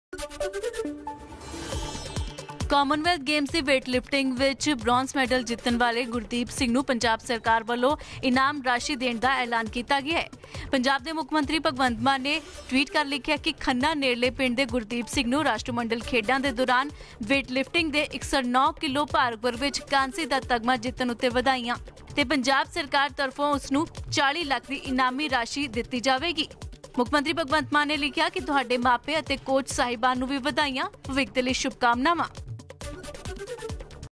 ਵੇਟ ਲਿਫਟਿੰਗ 'ਚ ਗੁਰਦੀਪ ਸਿੰਘ ਨੇ ਜਿੱਤਿਆ ਮੈਡਲ, ਮਾਨ ਸਰਕਾਰ ਨੇ ਦਿੱਤਾ 40 ਲੱਖ ਦਾ ਇਨਾਮ|ABP Sanjha |Punjab News
ਖੰਨਾ ਨੇੜਲੇ ਪਿੰਡ ਦੇ ਗੁਰਦੀਪ ਸਿੰਘ ਨੂੰ ਰਾਸ਼ਟਰ-ਮੰਡਲ ਖੇਡਾਂ ਦੌਰਾਨ ਵੇਟ ਲਿਫਟਿੰਗ ਦੇ 109kg ਭਾਰ ਵਰਗ ‘ਚ ਕਾਂਸੀ ਦਾ ਤਮਗਾ ਜਿੱਤਣ ‘ਤੇ ਵਧਾਈਆਂ…ਪੰਜਾਬ ਸਰਕਾਰ ਤਰਫੋਂ ₹40 ਲੱਖ ਦੀ ਇਨਾਮੀ ਰਾਸ਼ੀ ਗੁਰਦੀਪ ਨੂੰ ਦਿੱਤੀ ਜਾਵੇਗੀ